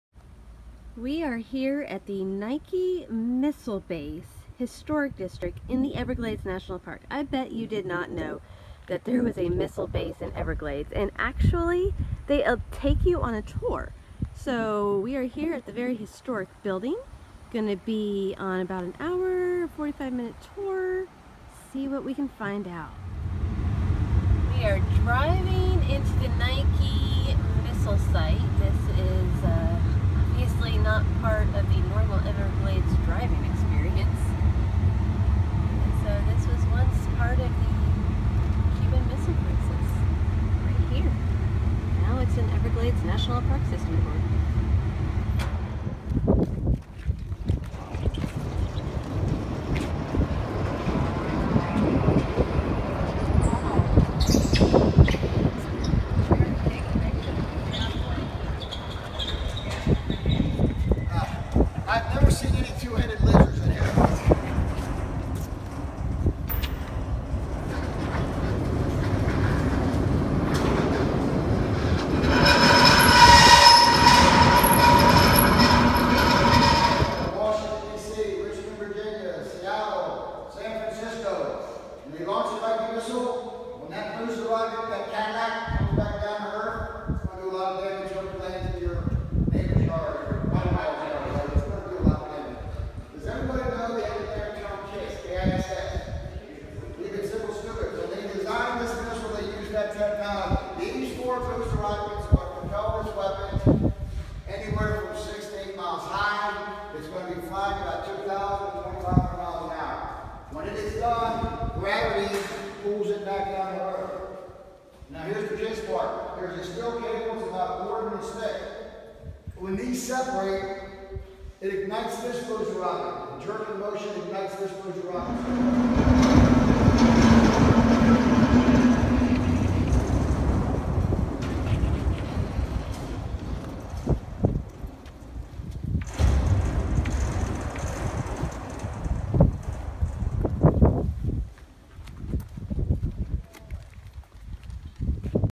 Nike Missile Base at Everglades National Park
Hidden in the Everglades National Park in Florida is a secret Nike Missile base. This Cold War relic housed missiles aimed at Cuba without anyone knowing. Take a free tour of this historic monument during a visit.